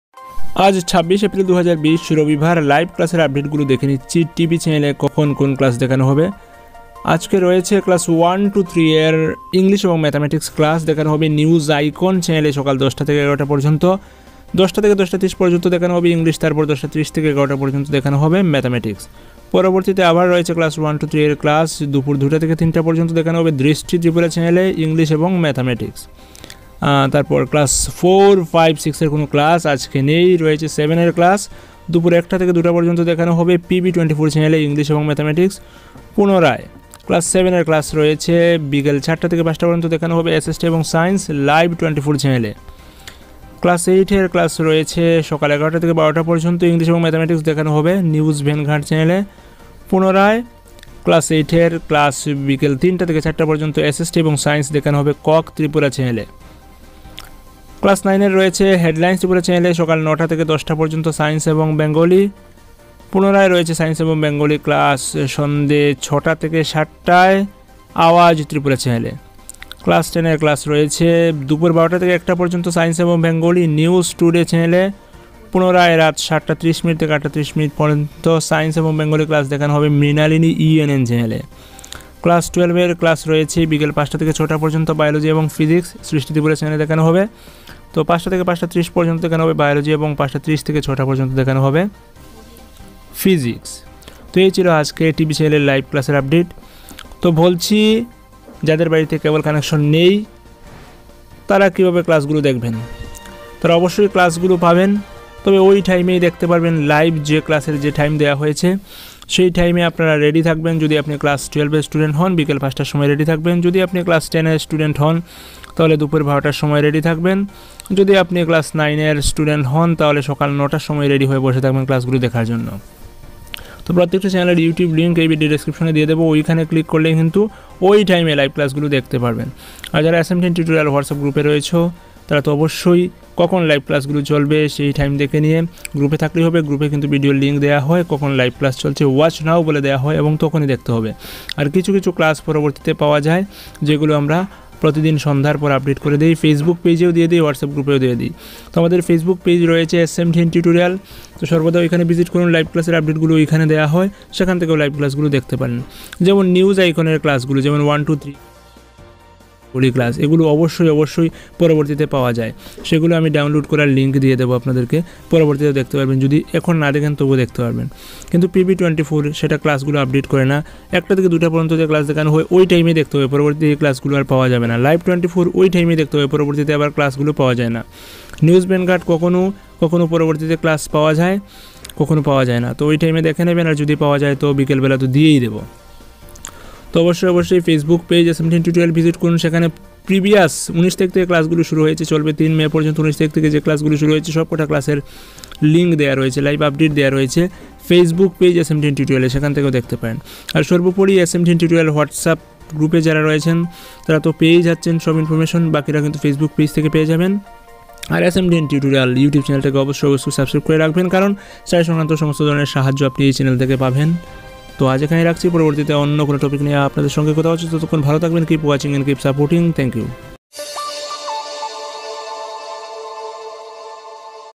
Live Class Update 26-04-2020 l Live Classes for 1,2,3,7,8,9,10,12 l Live Classes and Video Links l S
26/04/2020

All available Live Classes👇🏻

Classes 1,2,3

Class 12

English

(Hand Notes)

Class 10
Physics

Topic _ potential difference , Ohm's law and  Numerical example..

Teacher: Nabanita Chakraborty

Class 10
ELECTRICITY...
Topic -- তড়িৎপ্রবাহ, তড়িৎক্ষেত্র, তড়িৎবিভব

Class 12

English

Class 9

Science _ Bengali
Live classes Ongoing on Awaaz Tripura

6pm _ 7pm

26/04/2020
Sunday

LIVE Classes  Telecast Schedule

Classes : 1,2,3
Channel : News ICON
10:00am -11:00am
English / Mathematics

Classes : 1,2,3
Channel : Drishti Tripura
02:00pm -03:00pm
English / Mathematics

Class : 7
Channel : PB 24
01:00pm -02:00pm
English / Mathematics

Class : 7
Channel : LIVE 24
04:00pm -05:00pm
SST / Science

Class : 8
Channel : News Vanguard
11:00am -12:00pm
English / Mathematics

Class : 8
Channel : KOK Tripura
03:00pm -04:00pm
SST / Science

Class : 9
Channel : Headlines Tripura
09:00am -10:00am
Science / Bengali

Class : 9
Channel : Awaaz Tripura
06:00pm -07:00pm
Science / Bengali

Class : 10
Channel : News Today
12:00pm -01:00pm
Science / Bengali

Class : 10
Channel : Mrinalini ENN
07:30pm -08:30pm
Science / Bengali

Class : 12
Channel : Shrishti Tripura
05:00pm -06:00pm
Biology / Physics

With Regards
SMDN Tutorial